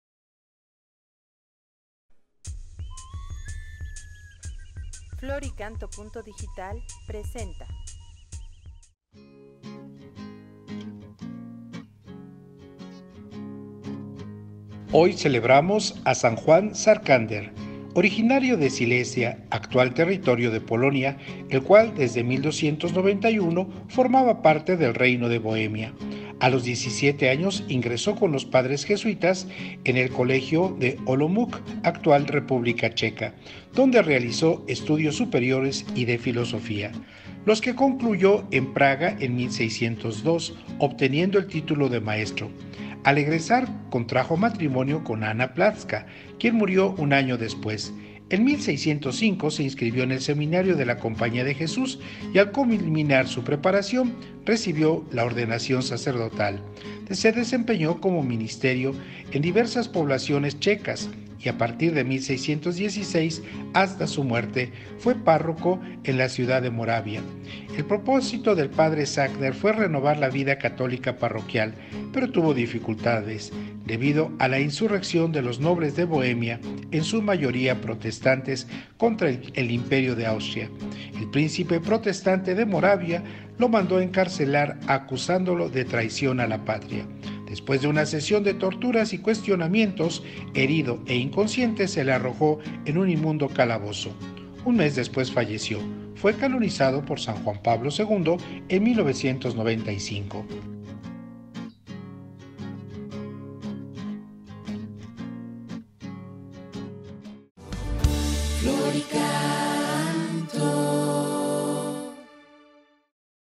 Marzo 17. San Juan Sarkander
Les presentamos la pequeña semblanza que el Padre Sergio Valdivia, asesor espiritual de Santa María Reina de la Paz. Flor y Canto nos comparte.